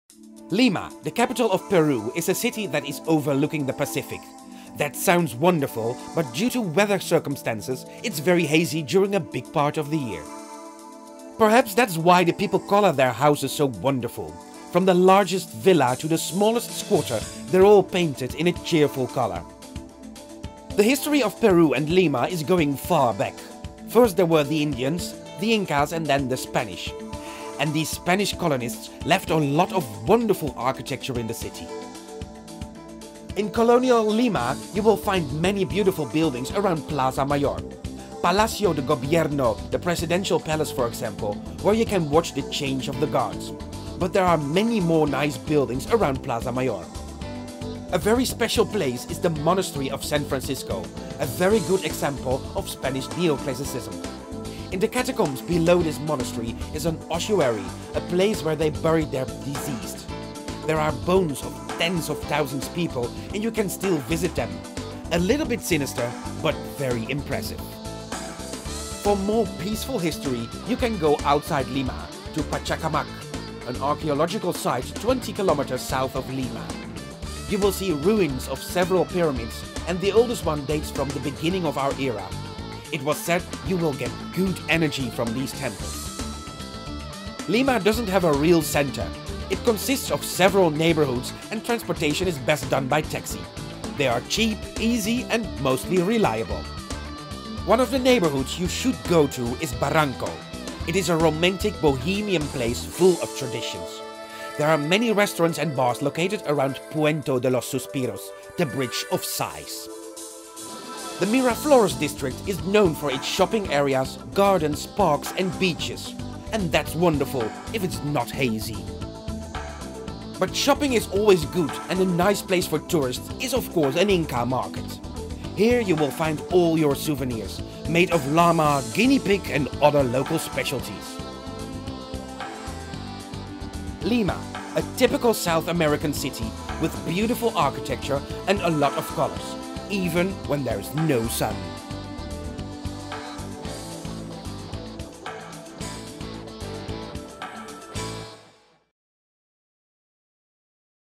IFLYtheworld.com Lima
Movie with travel tips about Lima, Peru, produced especially for IFLYtheworld.com, a travel guide with tips, movies, stories, information and fun, all contributed by airline crew.